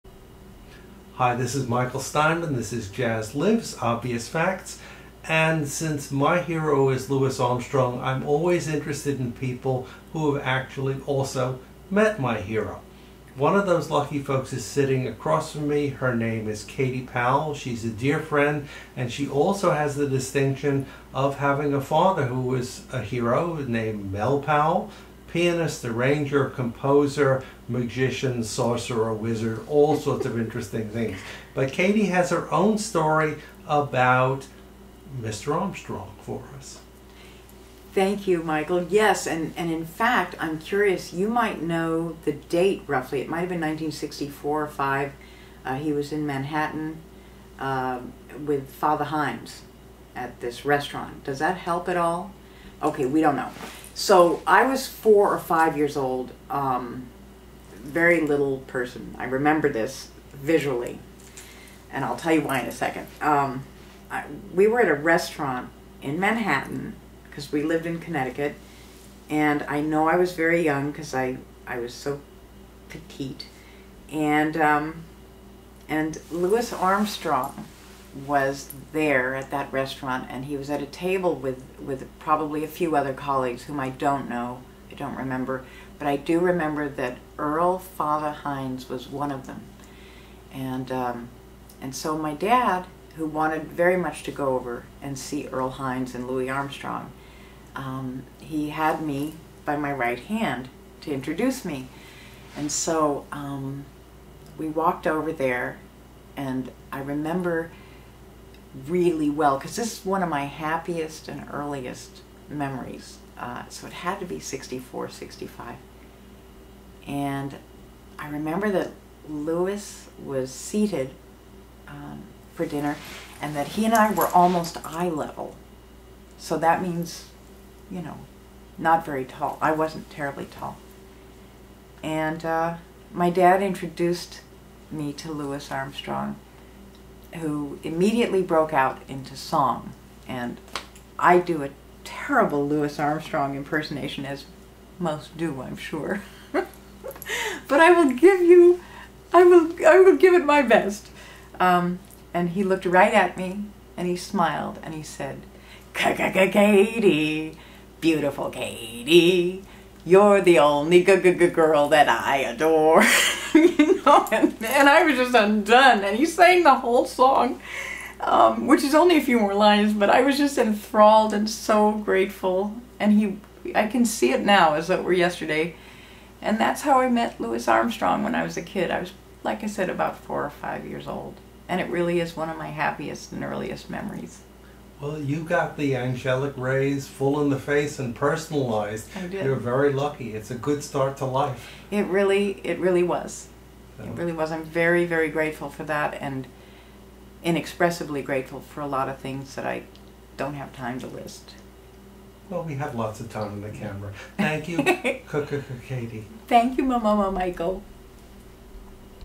KATI POWELL TELLS A TALE: MEETING LOUIS ARMSTRONG
Kathleen "Kati" Powell, actress, writer, raconteur, had singular parents: the pianist / composer / arranger Mel Powell and actress Martha Scott . . and she's inherited a great deal of their combined improvisatory splendor.  Here she shares the first of three narratives with us -- recorded on August 7, 2013, by Michael Steinman for JAZZ LIVES The New York Yankees, pork chops, Louis Armstrong, and more!